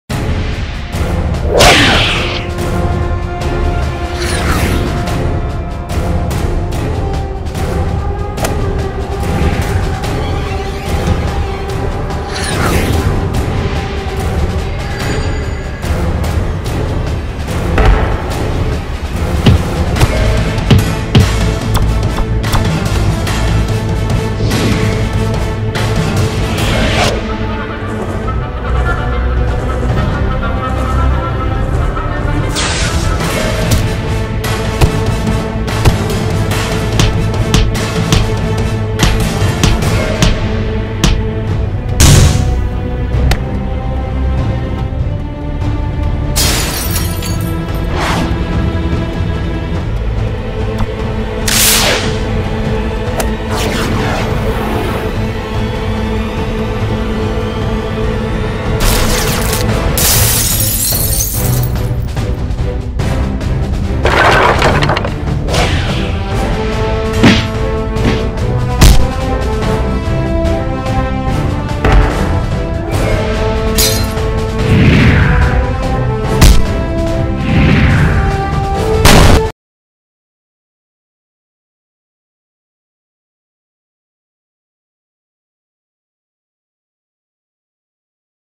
spiderman nicolette leo
For this video we had to add sound effects to this video. The most difficult part of this spiderman project was getting the timing right to match the sound effects. I enjoyed doing this while project, it was my favorite so far. This project will help me get my timing down right with the sound effects in the future with a movie.